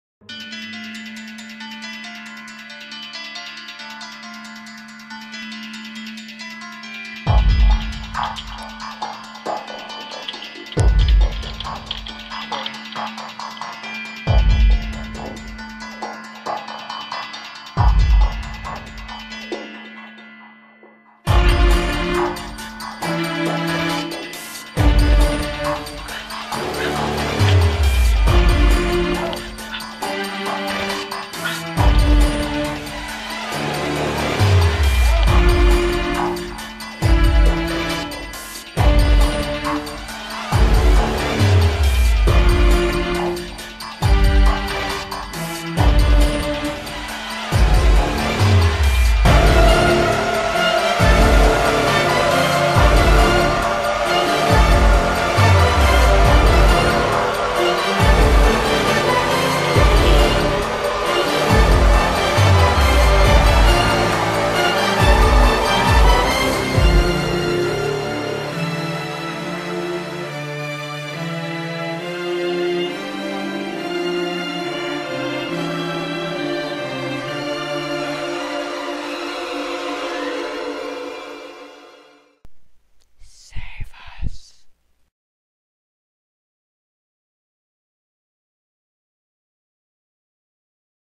The Third Eye
Unofficial trailer for the book The Third Eye by Lois Duncan.